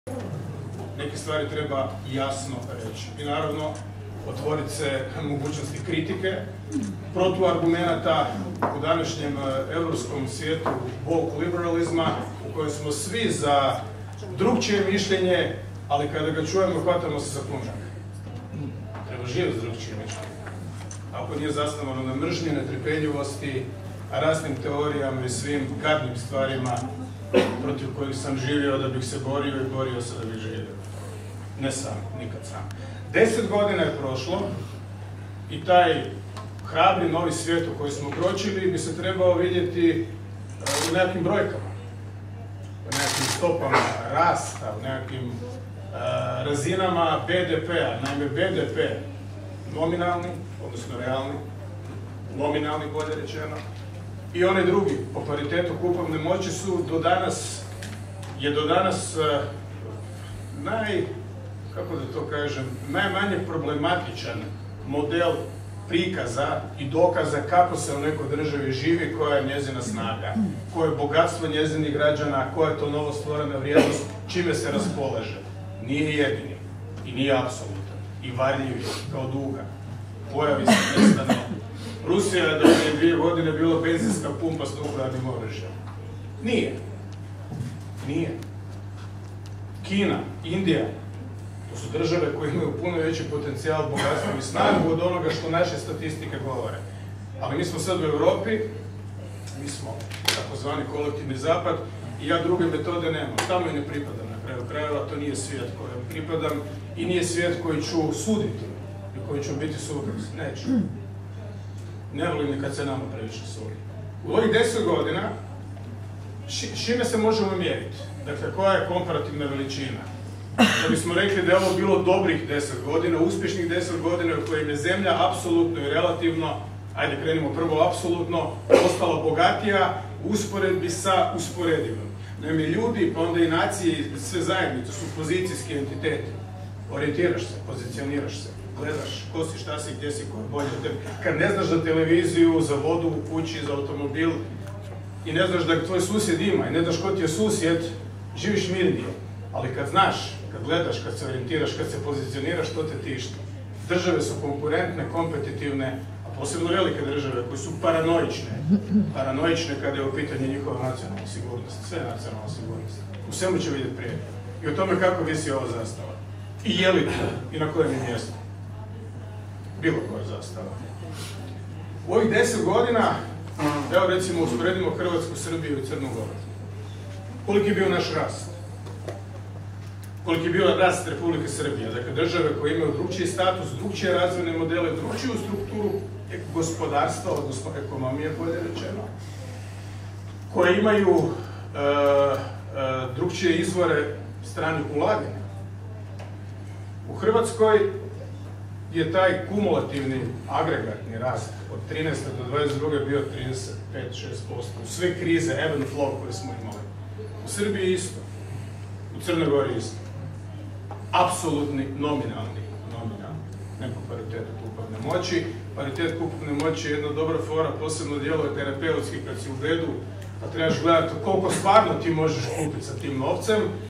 Milanović o BDP-u: "Varljiv je kao duga"
Predsjednik Republike Zoran Milanović sudjelovao je na konferenciji povodom obilježavanja 10 godina članstva Republike Hrvatske u Europskoj uniji.